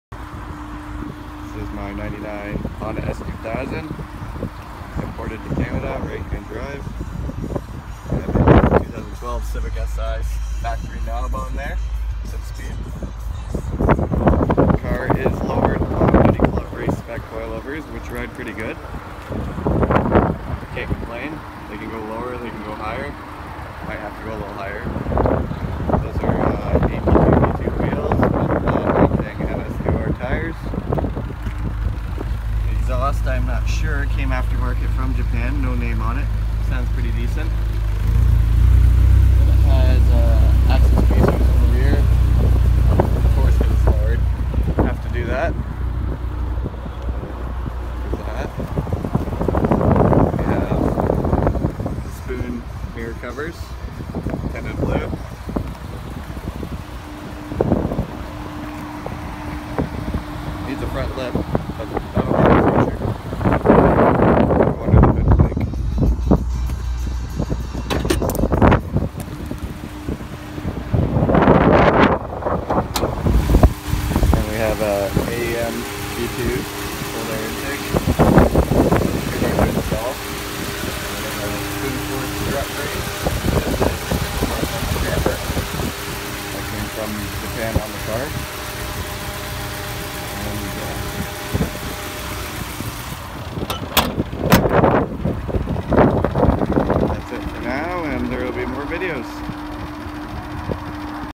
1999 JDM Honda S2000 right hand drive AP1
A quick walk around of my new car for my videos.